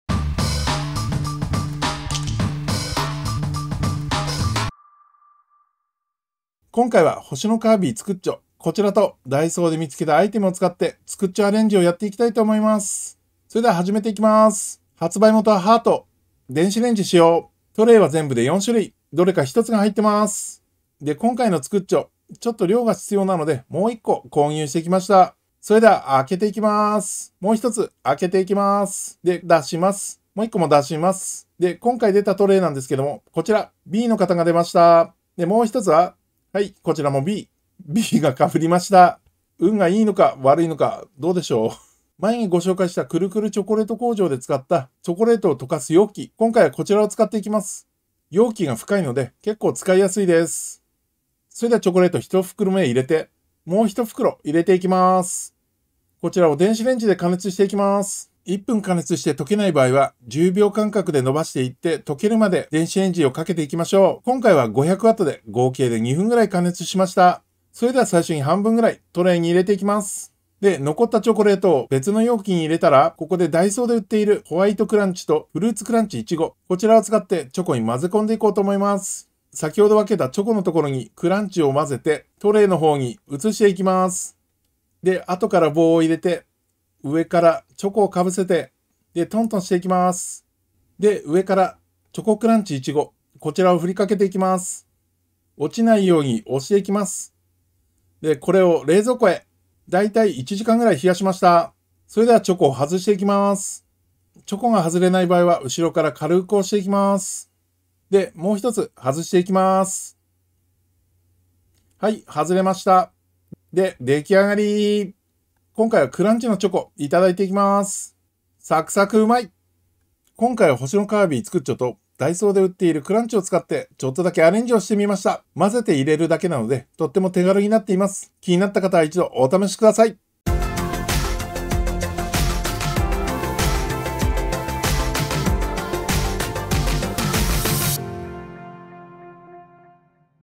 【Heart】星のカービィつくっチョ を使ってアレンジレシピ【1個248円】
スーパーで見つけた株式会社ハート「星のカービィ つくっチョ！」を使ってアレンジレシピを作ってみました。

つくっチョ！とDAISOで売っているクランチを使ってサクサク食感のチョコを作ってみました。

株式会社ハート オフィシャルサイト商品紹介

男女ともに人気の「星のカービィ」のスティックチョコが作れる商品です。
ピンク色のイチゴチョコは、カービィにぴったり♪
トレイは４種で、それぞれかわいいポーズのカービィデザインが楽しめます。

★イチゴチョコの加熱時の注意点★
【加熱後】
イチゴチョコは加熱後もかたまりに見えますが、よくかき混ぜて頂くことで溶けます。

【かき混ぜた後】
イチゴチョコは水分が非常に少ないチョコの為、粘り気が強いです。

※追加の加熱をすると、溶けにくくなるのでご注意ください。

ⓒNintendo / HAL Laboratory, lnc.

星のカービィつくっチョ - 株式会社ハート